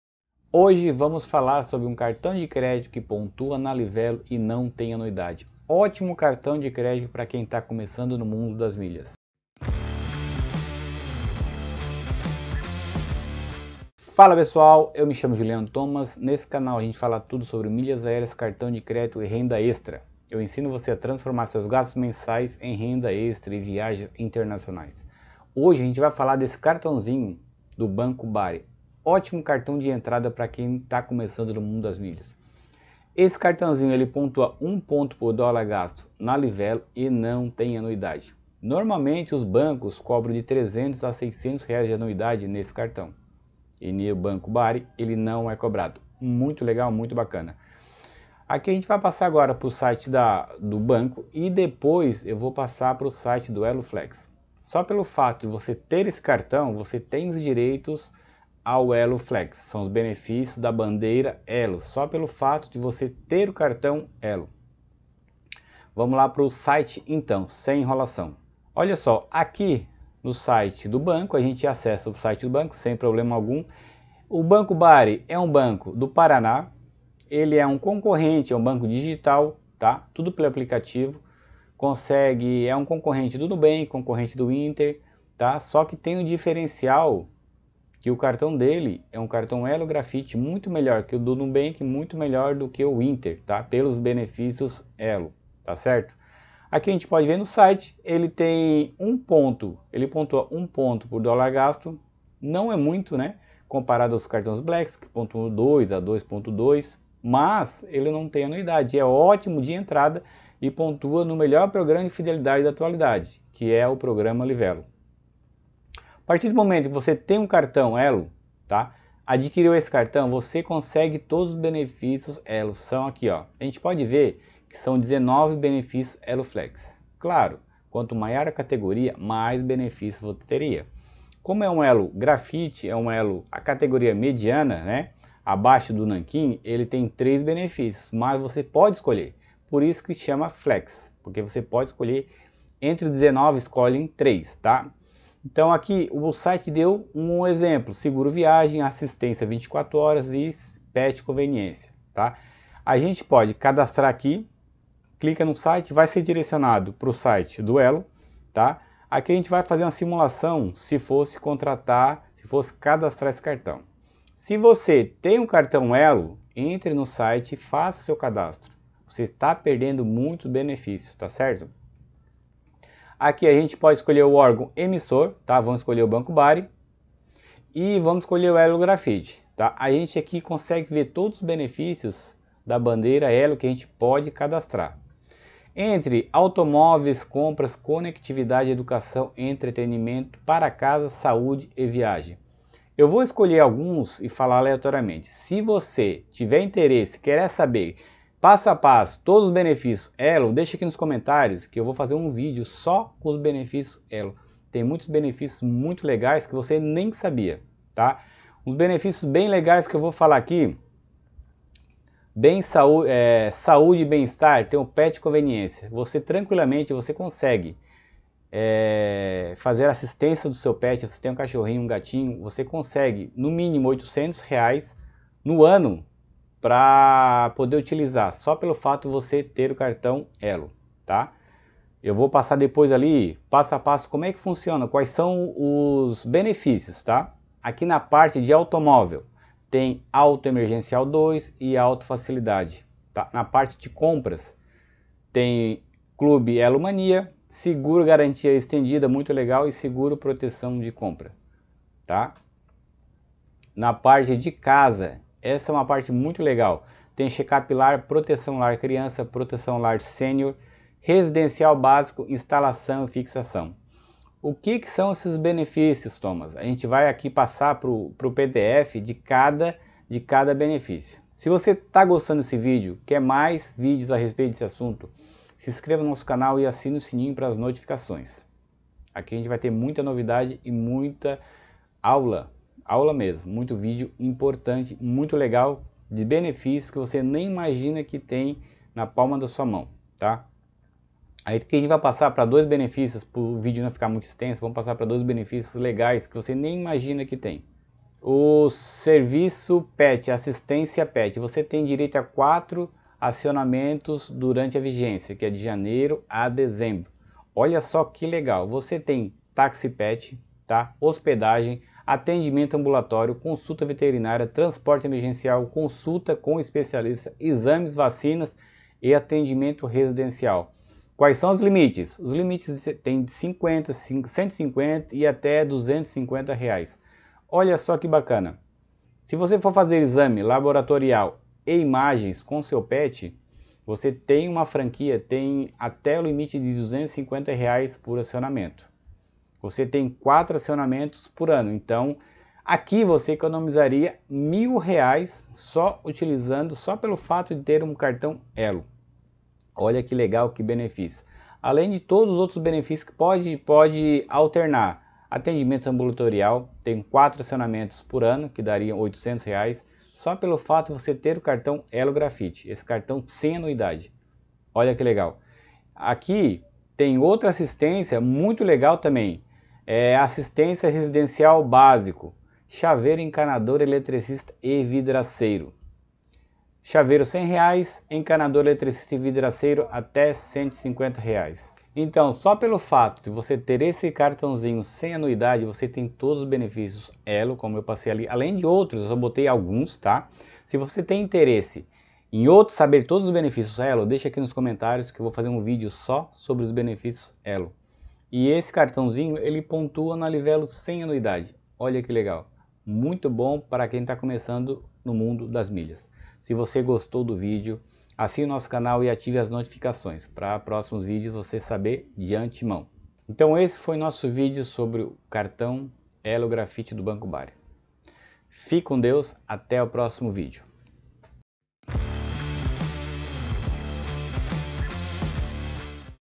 CARTÃO BARI: SEM ANUIDADE E PONTUA NA LIVELO 💰💳
Fala pessoal!! Hoje vamos falar de um cartão SEM ANUIDADE e que pontua na LIVELO, ótimo cartão de crédito para quem está começando no mundo das milhas..

O cartão do banco Bari é um ELO grafite e tem todos os benefícios do ELO FLEX, só escolher o melhor para você no site da ELO e partir para o abraço.

CURSO DOMINE AS MILHAS AÉREAS E TENHA RENDA EXTRA👇🏾👇🏾👇🏾